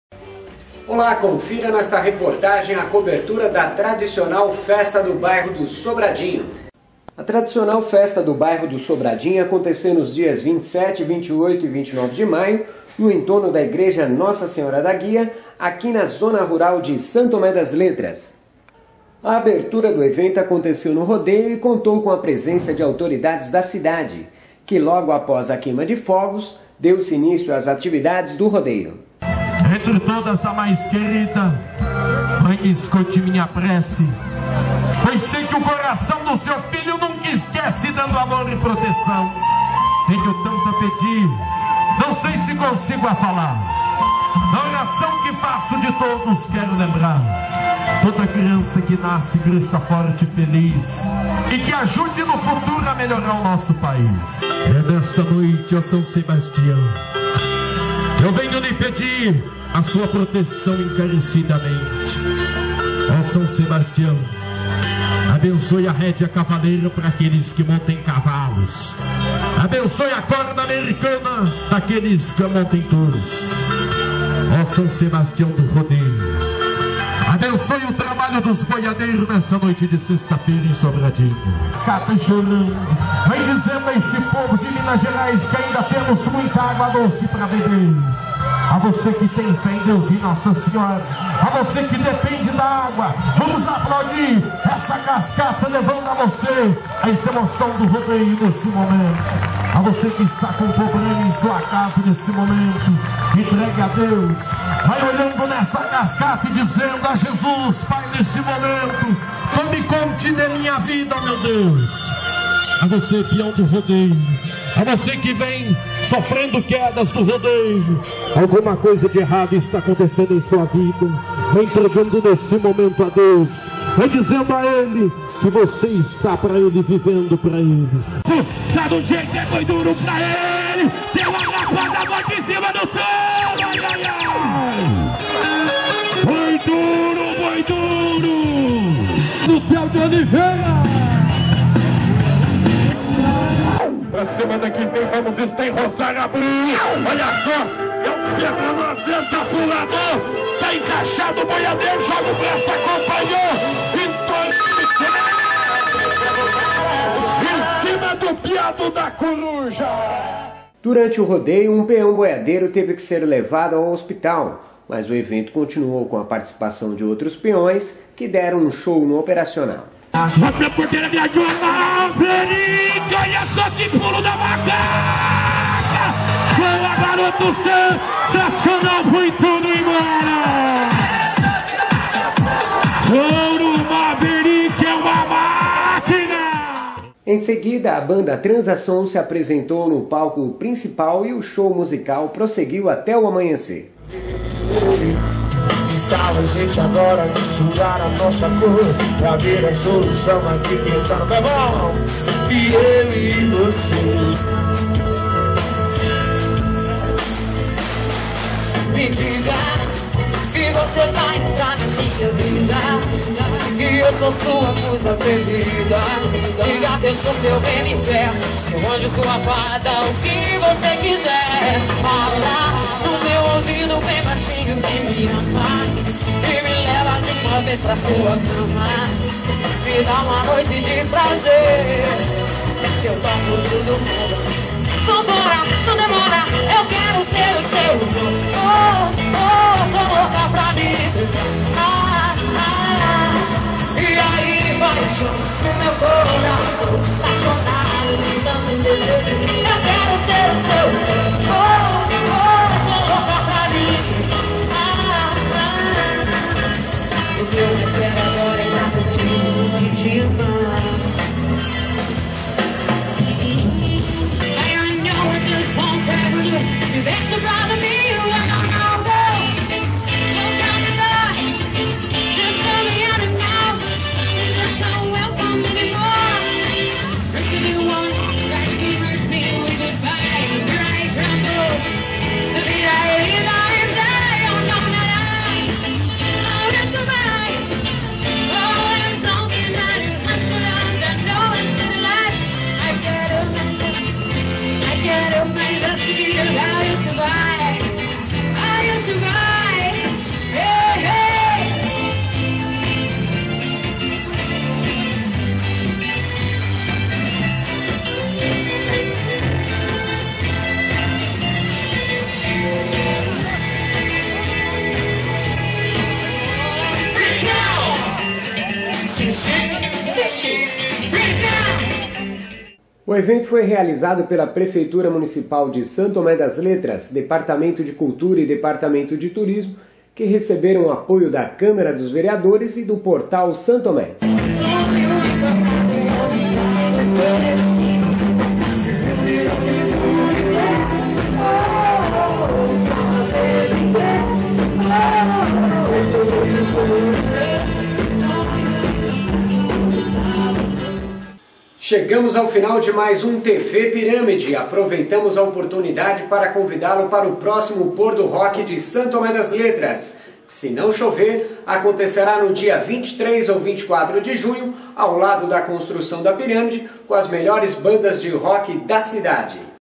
TV Pirâmide - Tradicional Festa do Sobradinho - São Tomé das Letras 2011
A Festa do bairro do Sobradinho aconteceu nos dias: 27, 28 e 29 de maio e contou com a presença de autoridades, moradores, turistas e grandes artistas do mundo da música. A abertura do evento foi no rodeio e contou com queima de fogos, festa do peão boiadeiro e grupo de danças que se apresentaram no palco.

Realização

Prefeitura Municipal de São Tomé das Letras
Departamento de Turismo
Departamento de Cultura

Promoção

JJB Produções

Cobertura

Portal São Thomé das Letras
TV pirâmide

Proibida a reprodução sem autorização